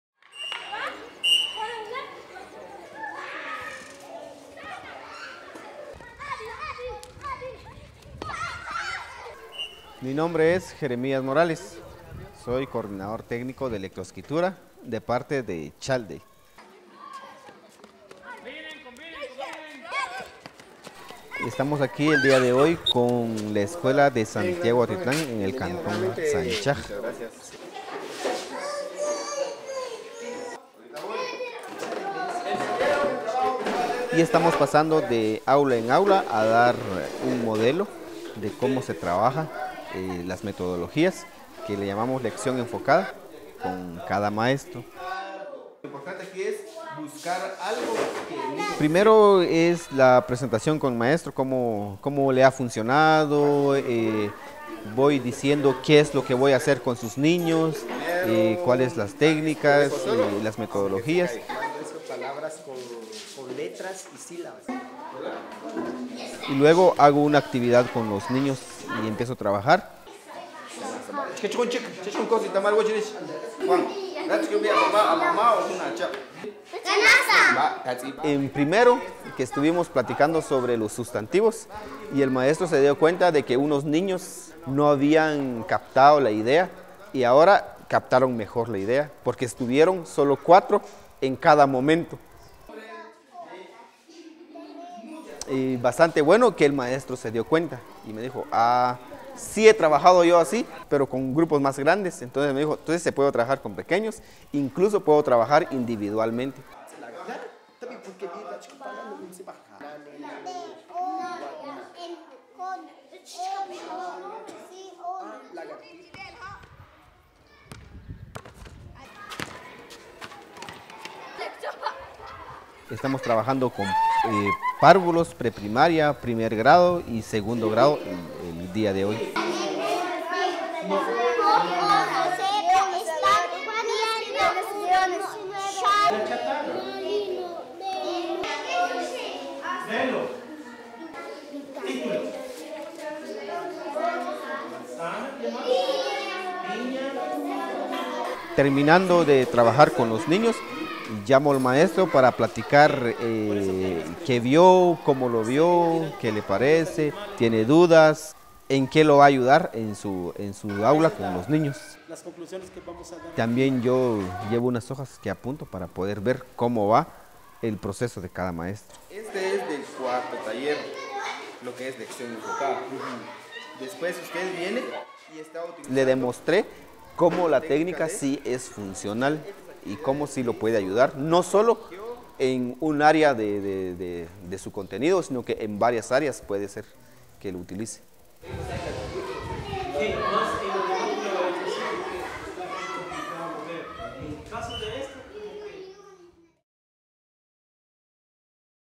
Bringing a Personal Touch to Teacher Training | Child Aid
One-on-one teacher followup sessions are at the heart of Child Aid's Reading For Life program and are what distinguishes it from other teacher training programs in Guatemala. Follow Literacy Trainer Jeremias Morales as he helps teachers in Santiago Atitlan, Guatemala apply the techniques and activities they learned in Reading for Life workshops in their classrooms.